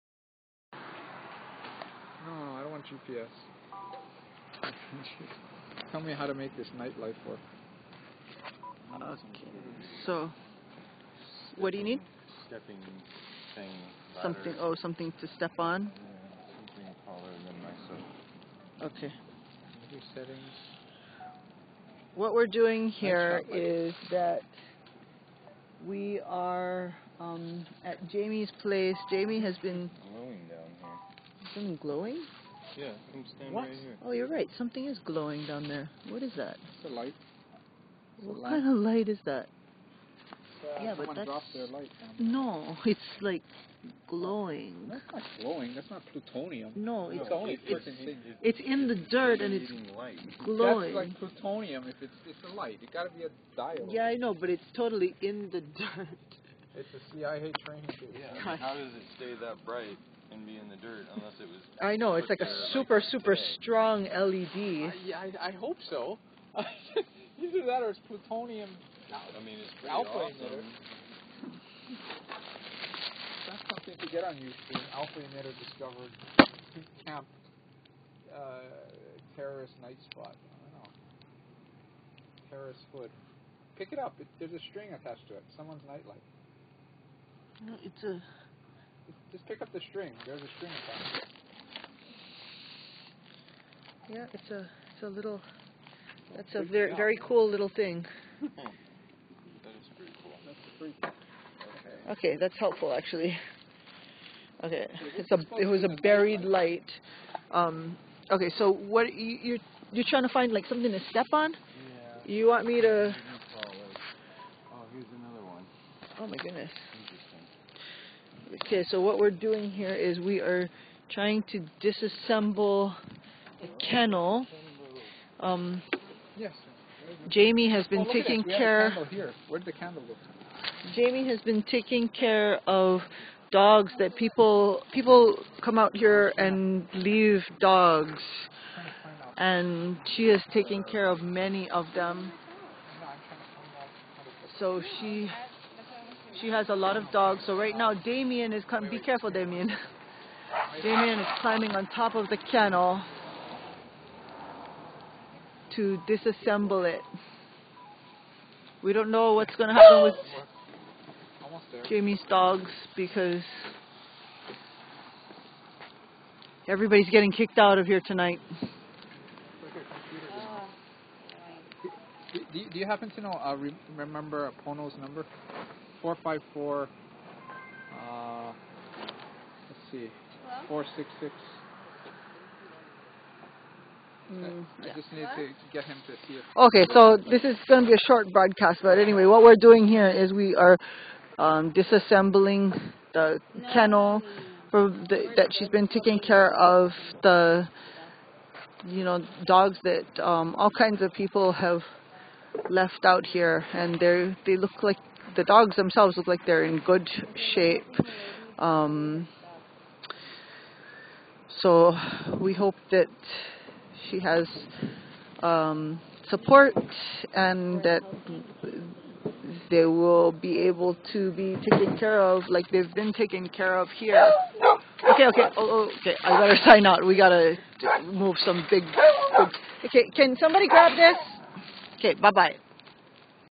‪Keaʻau: 10:22pm, discovery and deconstructing a kennel April 16, 2012
Light discovery. Effort also to disassemble a kennel.‪‪
Keaʻau Eviction‬‬ recorded live on April 16, 2012 10:22 pm, by AlohaRevolution